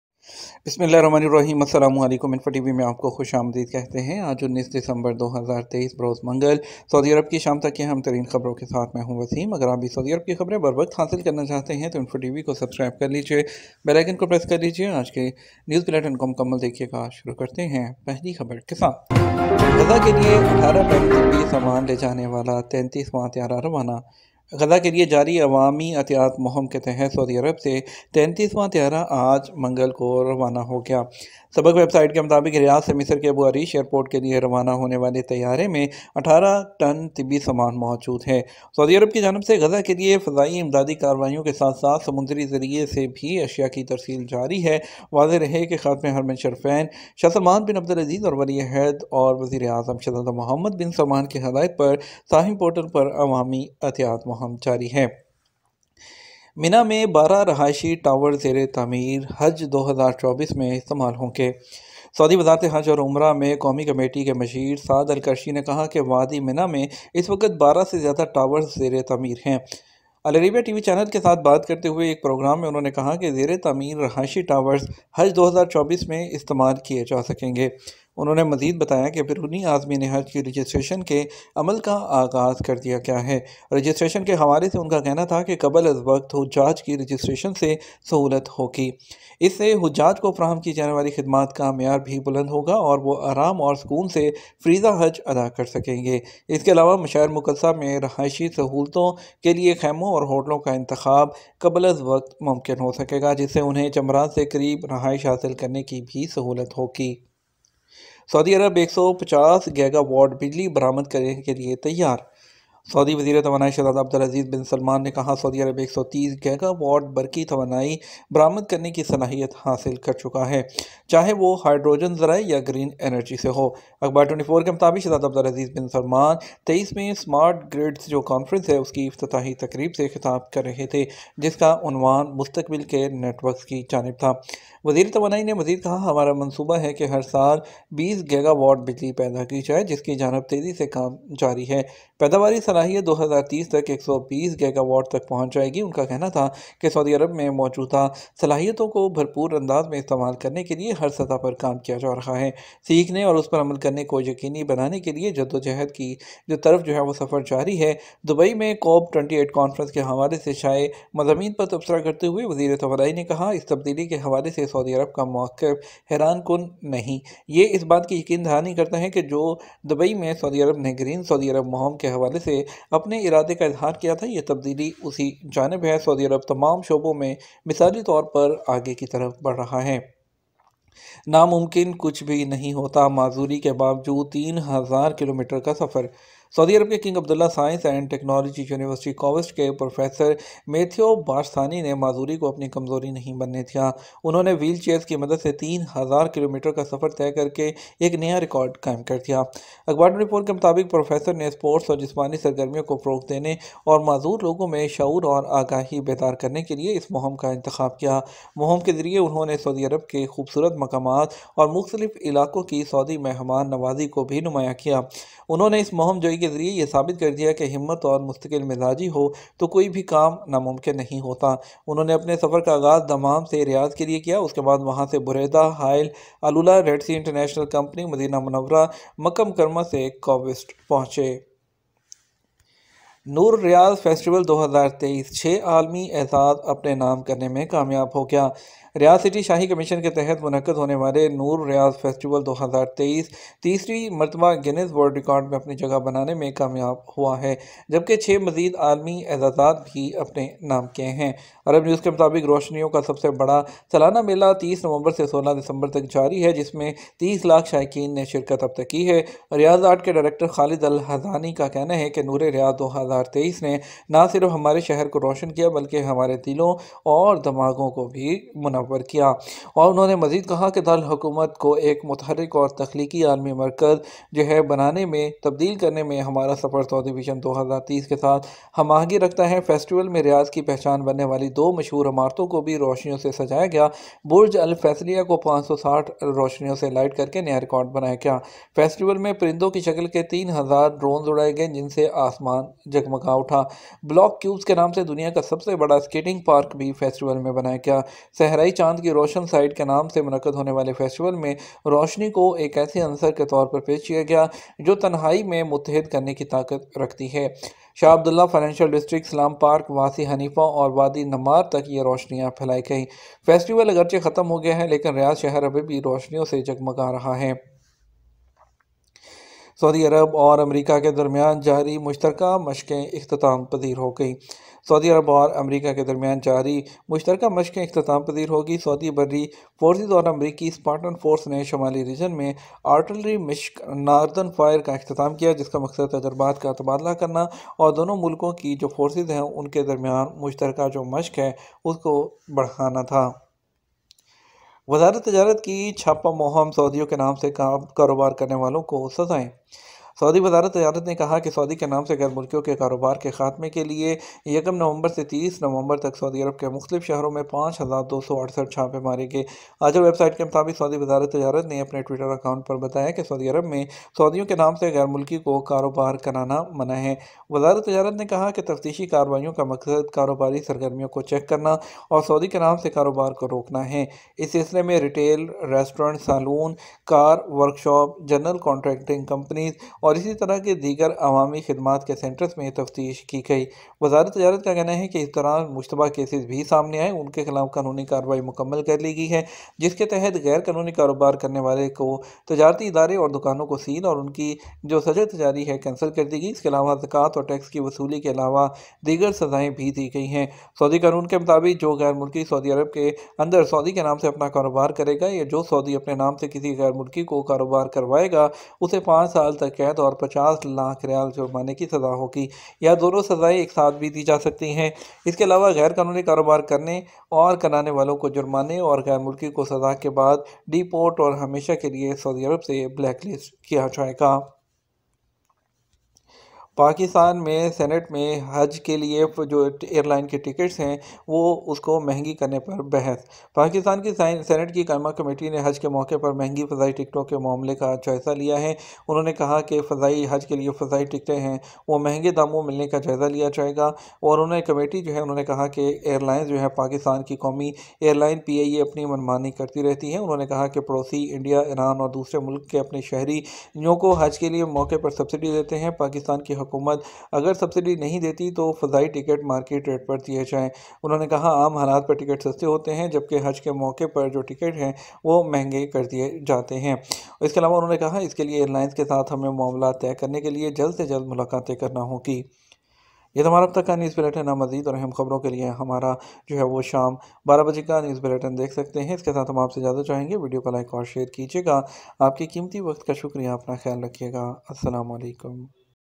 Breaking Saudi News Today Live In Urdu Hindi || Saudi Arabia ready to export 150 GW of Electricity
Breaking Saudi News Today Live In Urdu Hindi || Saudi Arabia ready to export 150 GW of ElectricityJoin Our Whatsapp Channel
saudi news today January 22 2023,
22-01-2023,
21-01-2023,
saudi news today January 21 2022,